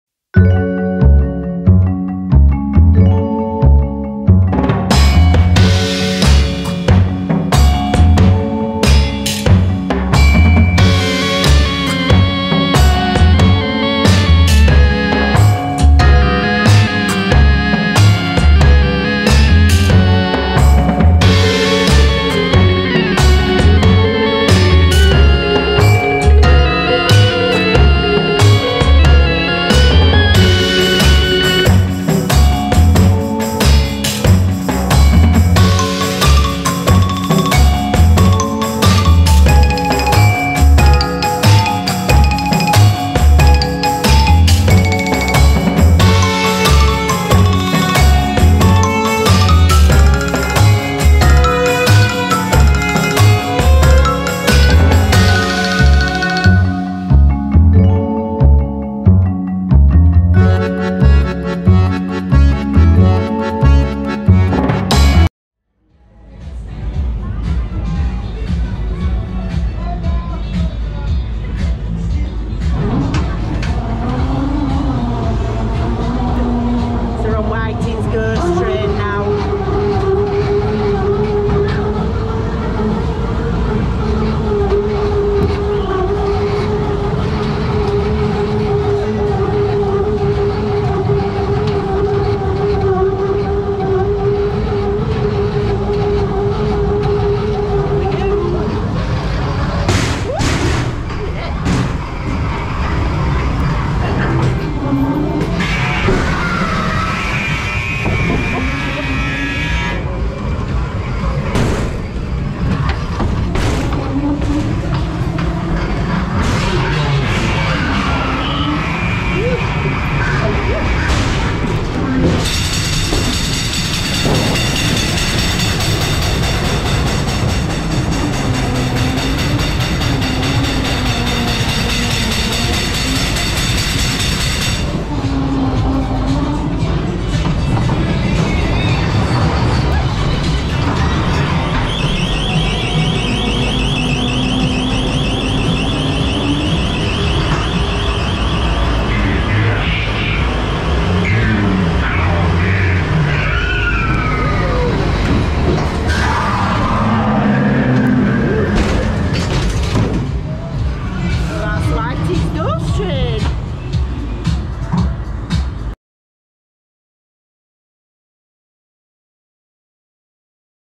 Jamie Whitings Ghost Train | on-ride POV | Ravensknowle Park Funfair | April 2022
Jamie Whitings Ghost Train On-ride pov

Join us for an on ride pov of Jamie Whitings Ghost train taken at Ravensknowle Funfair.

ABOUT US:-  My name is Lucas and I absolutely love theme parks, Disney and days out.  Joined with my lovely mum.